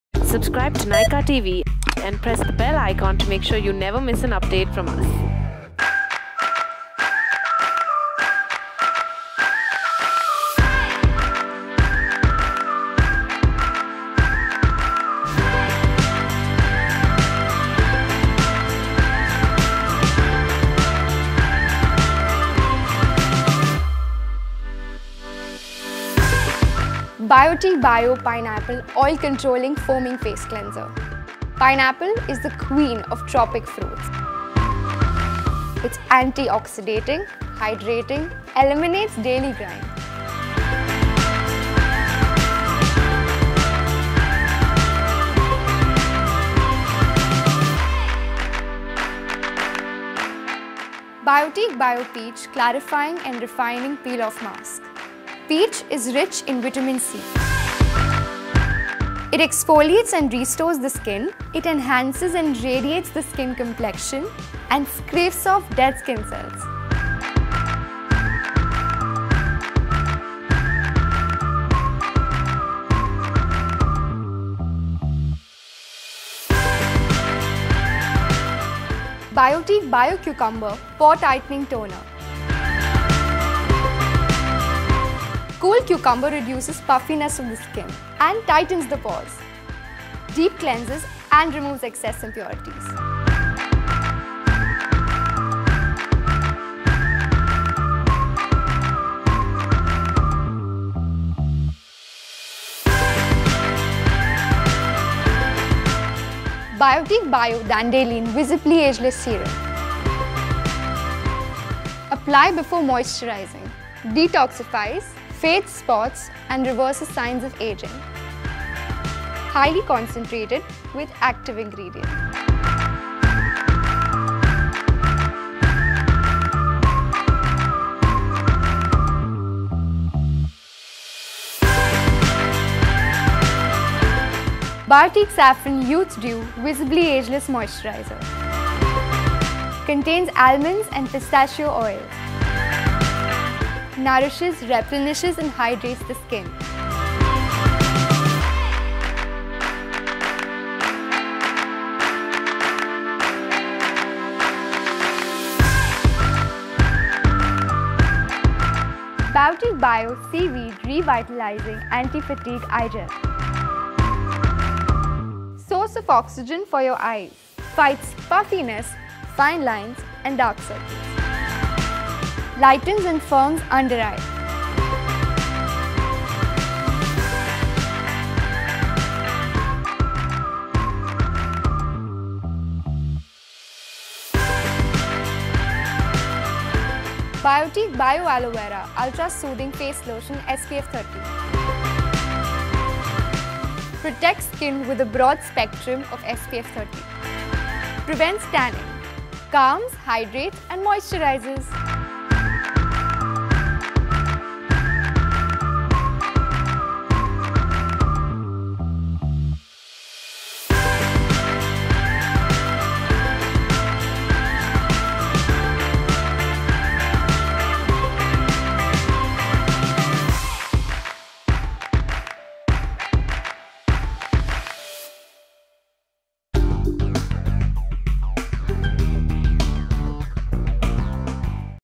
Make Monsoon a Happy Skin Season with Biotique and Nykaa
This Monsoon, pamper your skin with soothing, chemical-free, natural things. Biotique carries the goodness of Mother Nature, which helps you take care of your skin in the most natural way. See how each Biotique product helps you have a glowing, fresh-looking skin. In association with @Nykaa